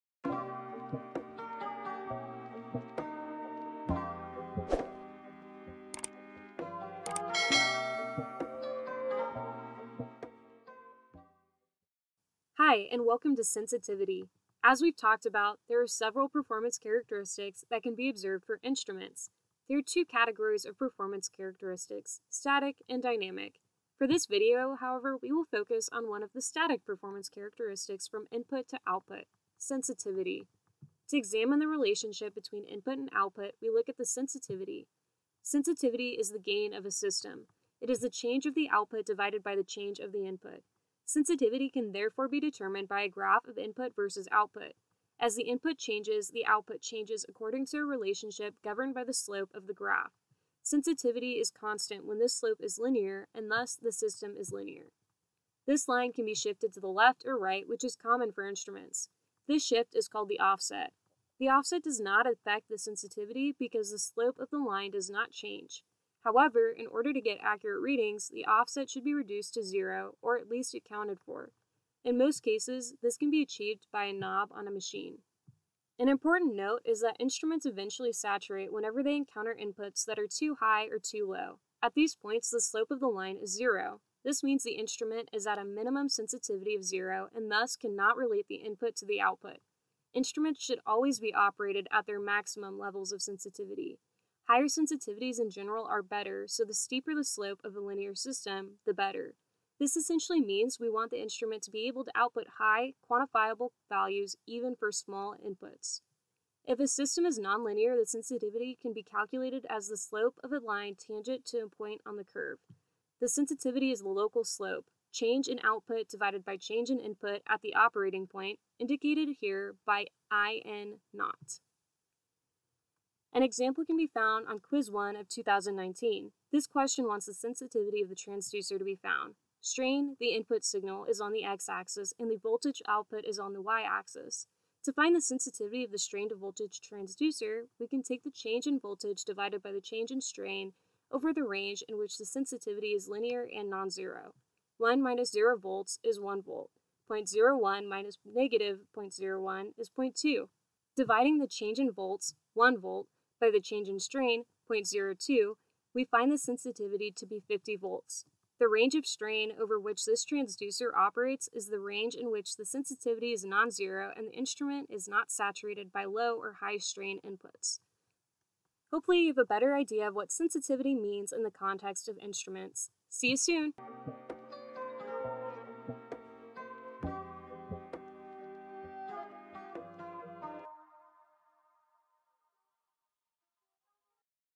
System (Sensor) Sensitivity with Practice Problem | Bioinstrumentation Design
BENG 186B: Principles of Bioinstrumentation Design (video 4)

Here we tackle the sensitivity of a system or sensor as the slope of the input-output relationship. Always remember, the higher the sensitivity the better! You always want your systems to operate in the region on highest sensitivity. In the meantime, for linear systems, the sensitivity is constant.

Want to give us some feedback so that we can better help you? Let us know what you think about the video here: